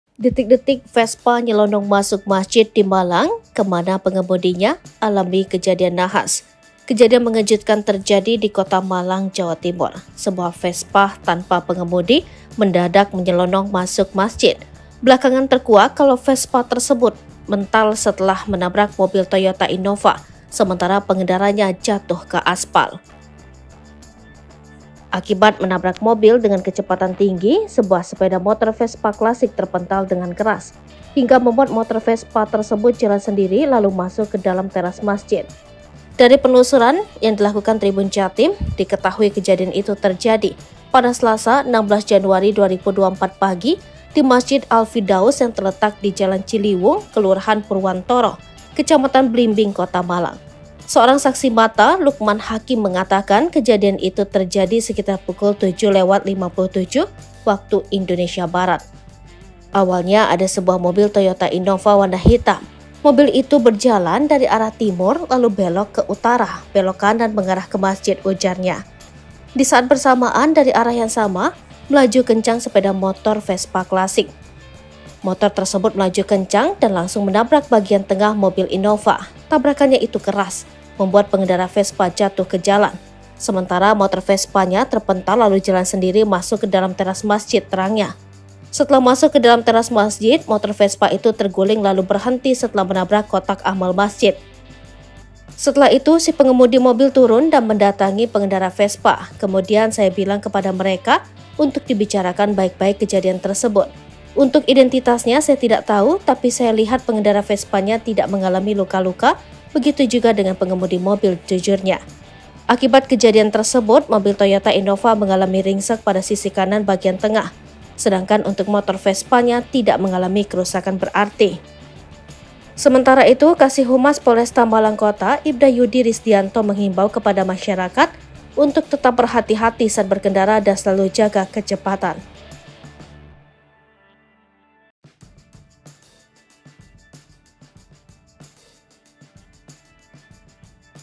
DETIK-DETIK Vespa Nyelonong Masuk Masjid di Malang, ke Mana Pengemudinya? Alami Kejadian Nahas
Kejadian mengejutkan terjadi di Kota Malang, Jawa Timur.

Sebuah vespa tanpa pengemudi mendadak nyelonong masuk masjid.

Belakangan terkuak kalau vespa tersebut mental setelah menabrak mobil Toyota Innova, sementara pengendaranya jatuh ke aspal.
Lihat informasi terkini bersama kami, di:

Palembang.tribunnews.com

Sumber Video: Tribun Jatim
Editor Video: Sarah
Vo: ulid